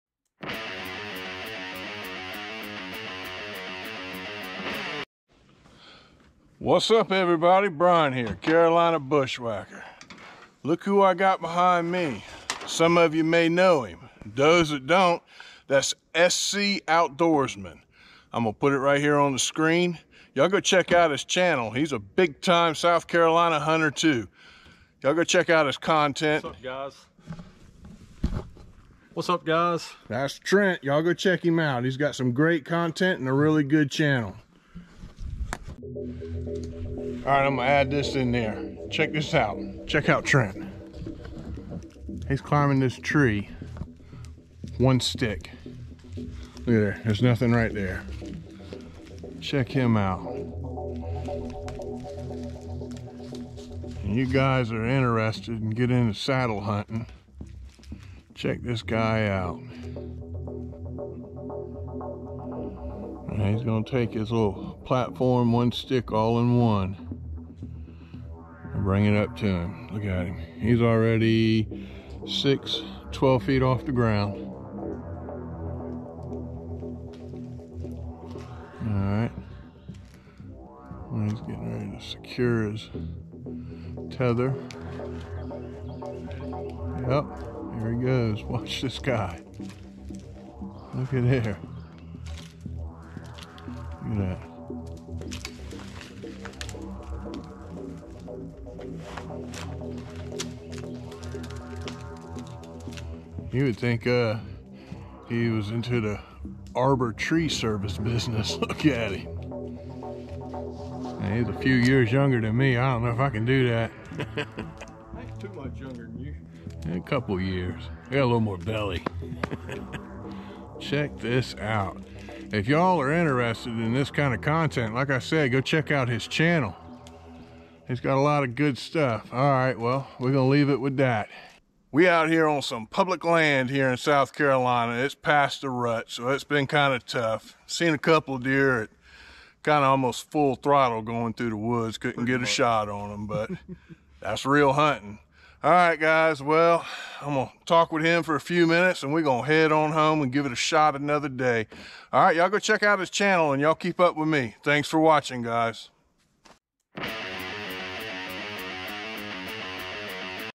ONE STICK SADDLE HUNTING WITH FELLOW YOUTUBER (SC OUTDOORSMAN)
Me (Carolina Bushwacker) and Trent (SC Outdoorsman) get together to do a little public land hunt here in South Carolina. He also shows me how he climbs a tree for saddle hunting with just one stick.